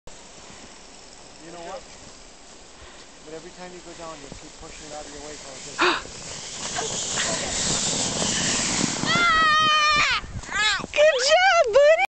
sledding
we took owen sledding for his first time.  he took the hill all by himself